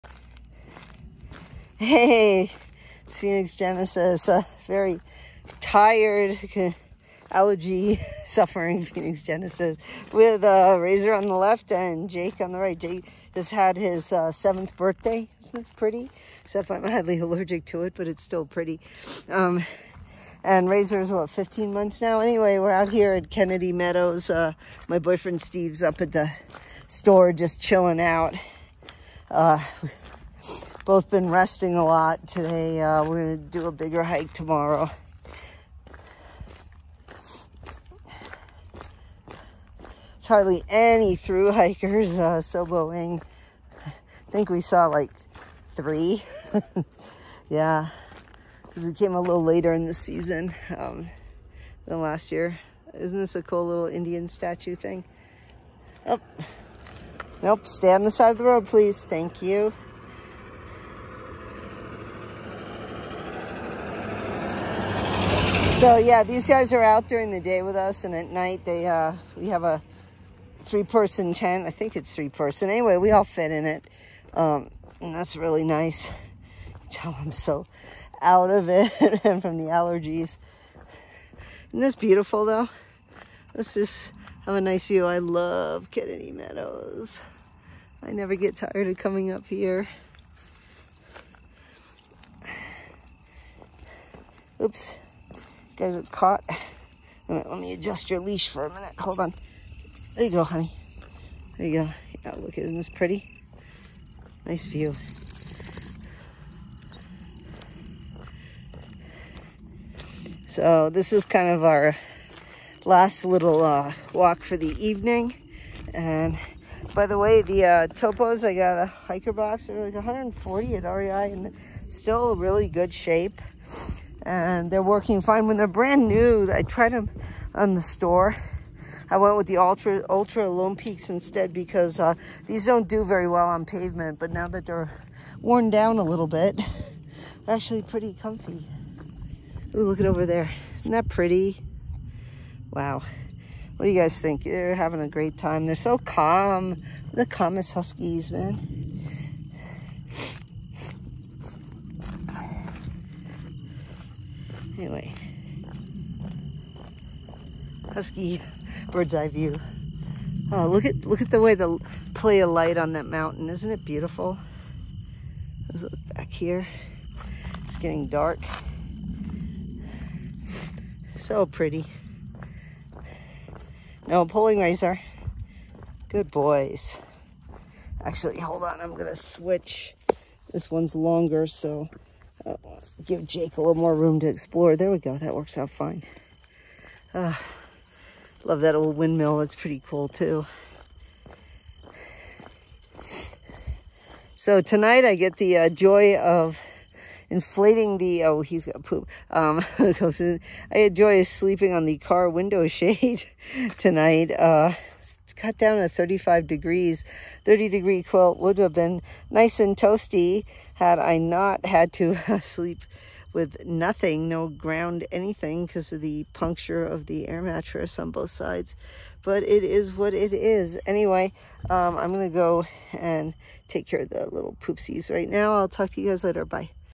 PCT 2023 - A Nice Sunset Stroll in the Sierra Nevadas with My 2 Siberian Huskies
We only did 5 miles for this walk, but they had a blast last night. We got up early this morning and hit the Pacific Crest Trail. My service dogs had a blast living their best call of the wild moments at Kennedy Meadows.